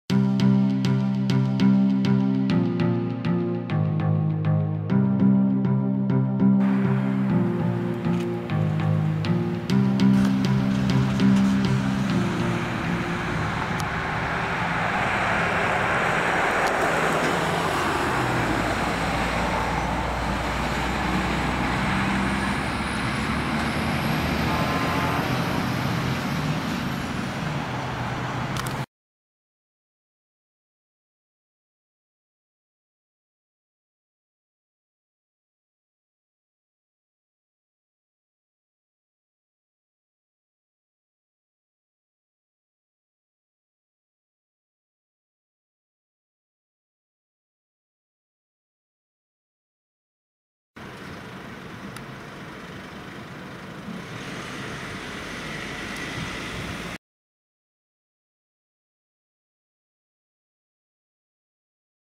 Mercedes Erlkönig AMG GT 4-door 73e * Unterschiedliche Spoiler * X290 different spoilers * 4K SPY
Mercedes Erlkönig: AMG GT 4-Türer 73e Coupé, AMG testet verschiedene Heckspoiler-Varianten (möglicherweise Facelift?)
Mercedes Prototypes: AMG GT 4-door 73e Coupé, AMG is testing various spoiler variants (possibly Facelift?)

(99 - schau)